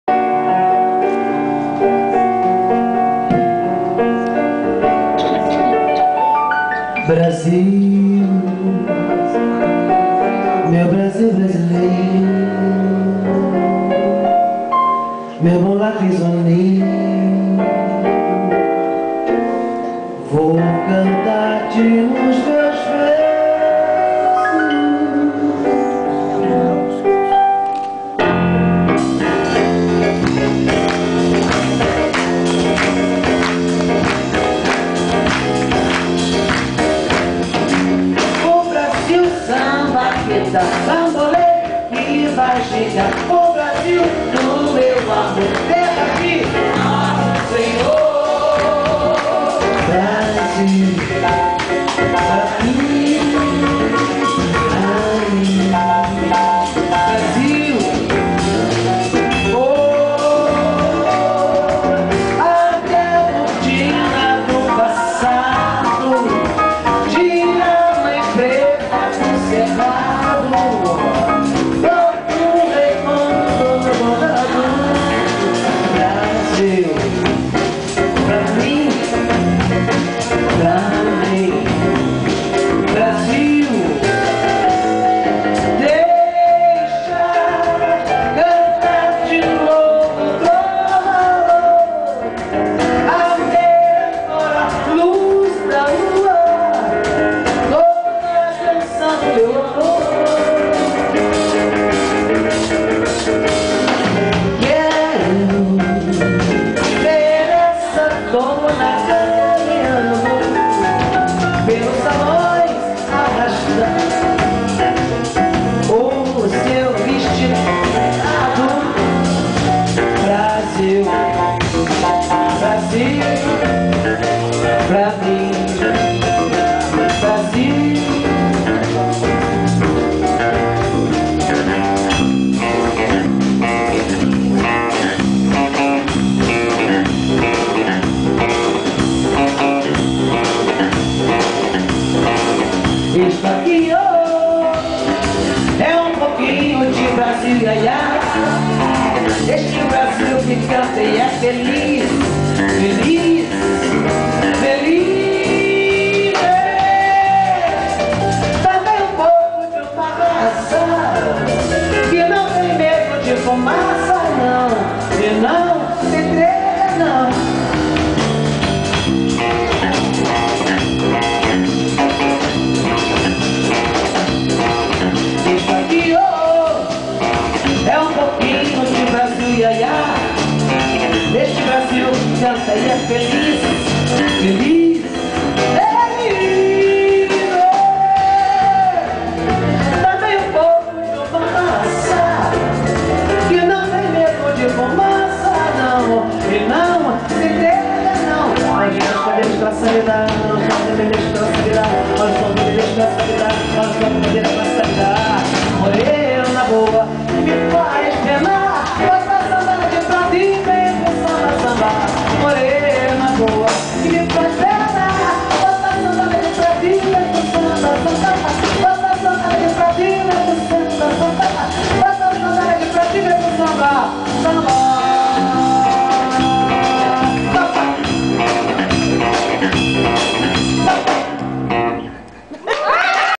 Eliana Pittman no Teatro do Centro Cultural Light
Show da cantora Eliana Pittman realizado em 19/08/2008 dentro do Projeto Terças Musicais Light, produzido pela Canal, Comunicação e Cultura e patrocinado pela Light Serviços de Eletricidade. Maiores informações